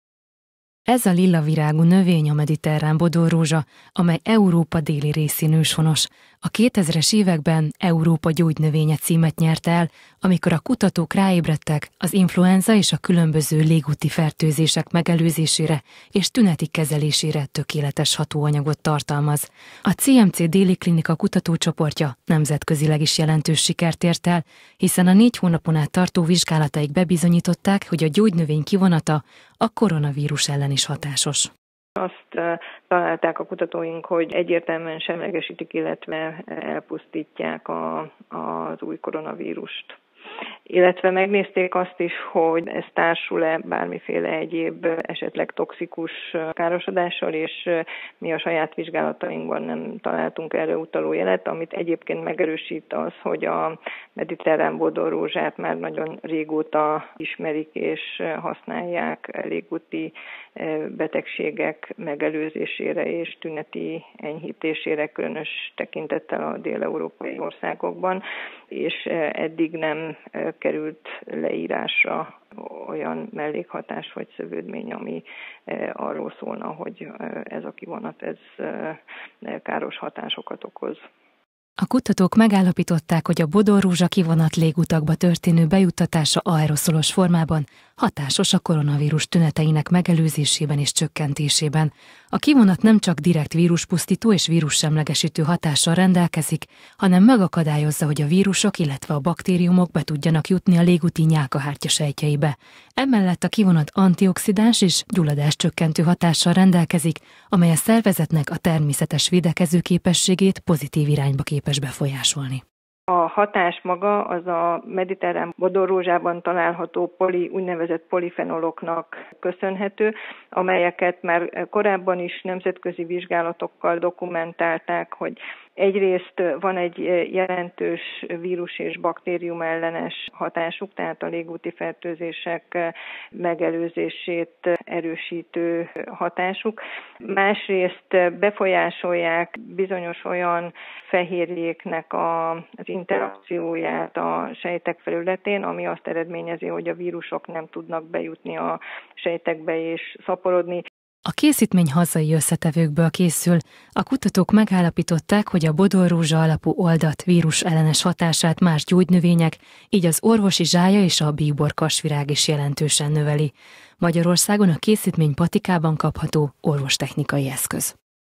A bodorrózsa lehet a fegyver a koronavírus ellen - Kölcsey Tv
Nemzetközileg is jelentős eredményt értek el magyar kutatók. Egy fővárosi klinika orvosai vizsgálataikkal igazolták, hogy az Európa déli részén őshonos  mediterrán bodorrózsa-kivonat gátolja a koronavírus szaporodását, valamint pozitív irányban befolyásolja a vírusfertőzés miatt kialakult immunológiai reakciókat is.